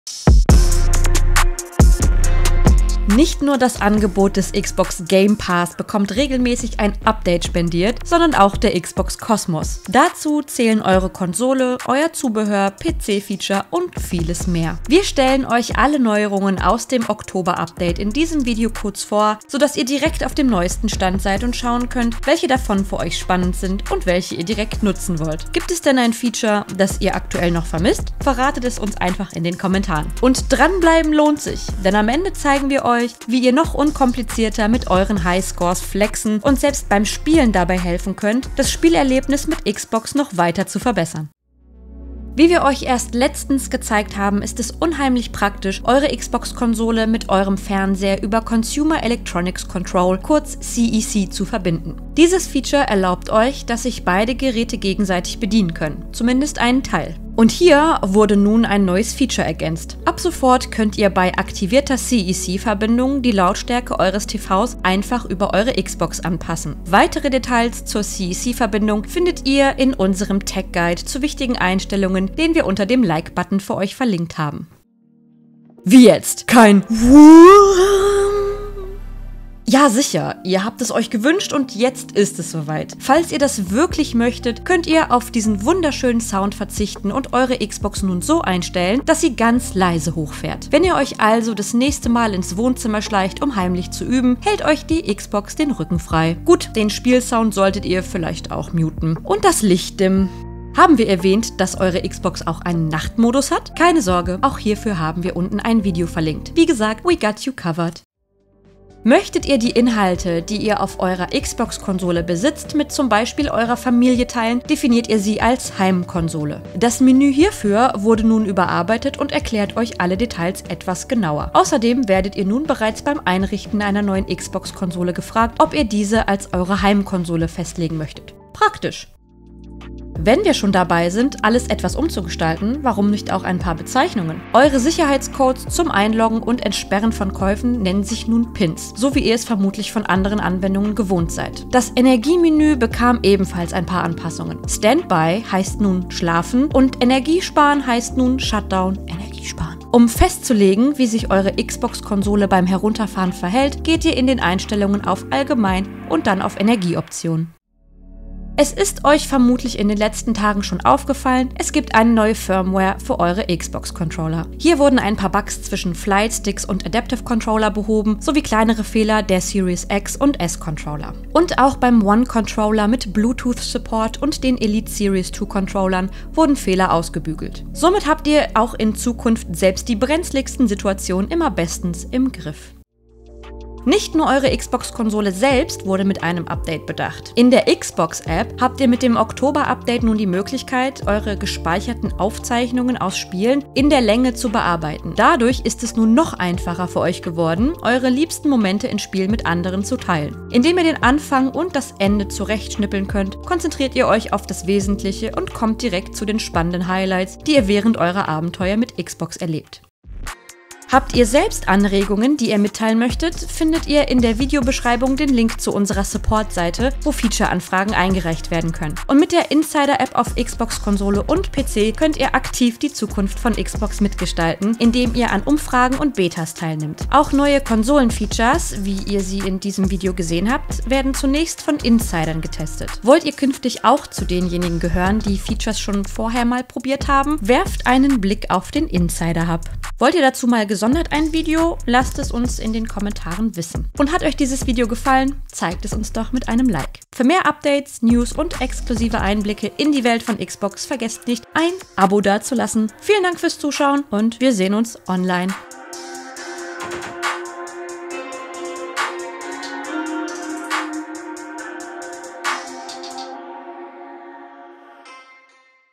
Eure Konsole kann jetzt noch mehr!
Auch in diesem Monat gab es für eure Next Gen-Konsolen wieder klasse Neuigkeiten. Mit dem Xbox-Update im Oktober kam eines der meist geforderten Features von der Community auf eure Konsole: Ab sofort könnt ihr nämlich eure Konsole komplett leise und ohne jeglichen Sound starten.

Welche Features sonst noch im neuesten Update stecken, erfahrt ihr im Video.

0:00 Intro
0:42 Neue CEC-Option: Lautstärke des TVs ändern
1:15 leises Hochfahren der Xbox
1:49 Neue Darstellung des Heimkonsolen-Menüs
2:10 Neue Bezeichnung in Menüs
2:38 Neue Controller Firmware
3:03 Clip-Anpassung in der Xbox-App
3:40 Xbox Insider-App